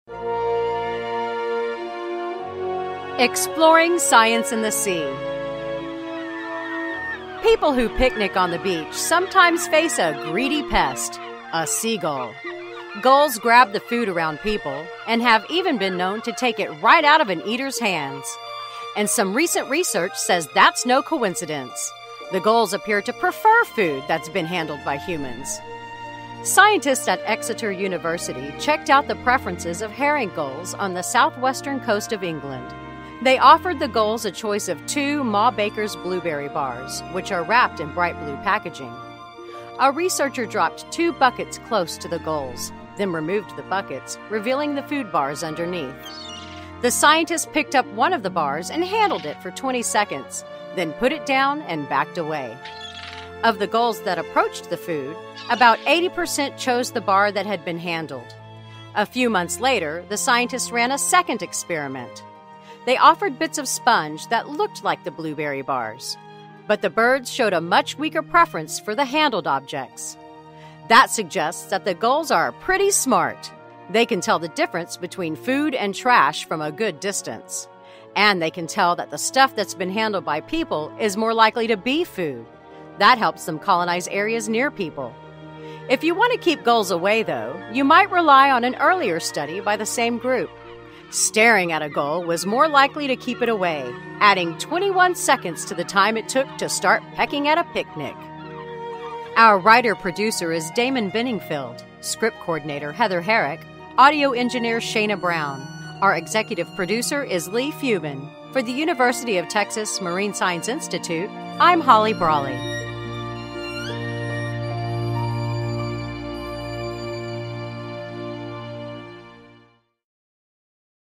Greedy Gullys
People who picnic on the beach sometimes face a greedy pest: a seagull. Gulls grab the food around people -- and have even been known to take it right out of an eater’s hands. And some recent research says that’s no coincidence -- the gulls appear to prefer food that’s been handled by humans.Scientists at Exeter University checked out the preferences of herring gulls on the southwestern coast of England. They offered the gulls a choice of two Ma Baker’s blueberry bars, which are wrapped in bright blue packaging.A researcher dropped two buckets close to the gulls, then removed the buckets, revealing the food bars underneath. The scientist picked up one of the bars and handled it for 20 seconds, then put it down and backed away. Of the gulls that approached the food, about 80 percent chose the bar that had been handled.A few months later, the scientists ran a second experiment. They offered bits of sponge that looked like the blueberry bars. But the birds showed a much weaker preference for the handled objects.That suggests that the gulls are pretty smart. They can tell the difference between food and trash from a good distance. And they can tell that the stuff that’s been handled by people is more likely to be food. That helps them colonize areas near people.If you want to keep gulls away, though, you might rely on an earlier study by the same group. Staring at a gull was more likely to keep it away -- adding 21 seconds to the time it took to start pecking at a picnic.